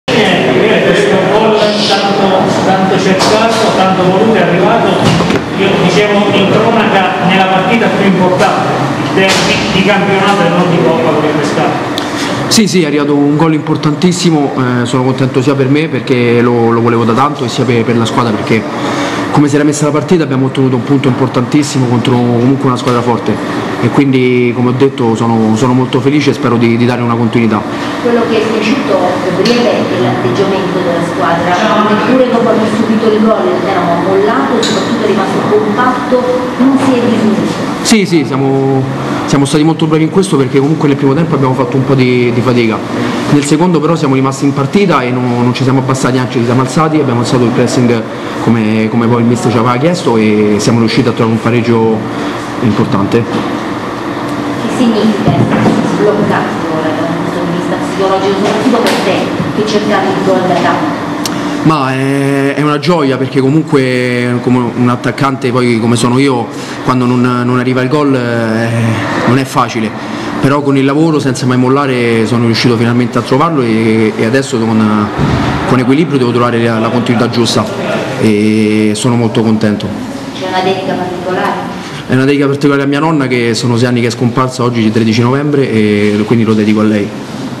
Pescara - Teramo 1-1: Gabriele Bernardotto
Conferenza stampa del centravanti del Teramo autore del gol che ha permesso ai biancorossi di acciuffare il pareggio contro il Pescara